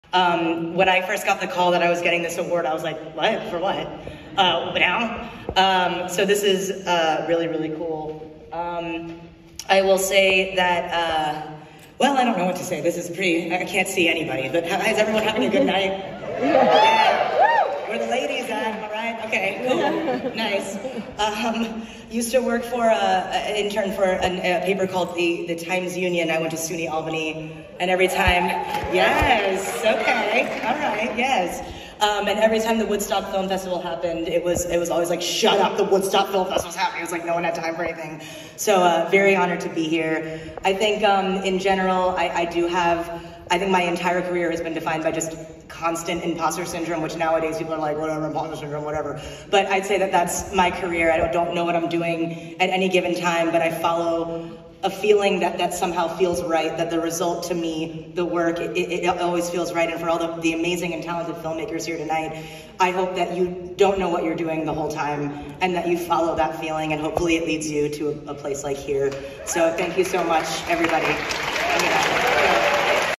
Awkwafina accepts the Woodstock Film Festival Transcendent Talent Award
Awkwafina accepts the Woodstock Film Festival Transcendent Talent Award at the Maverick Awards ceremony at the Ulster Performing Arts Center in Kingston, NY, on Saturday, Oct. 1, 2022.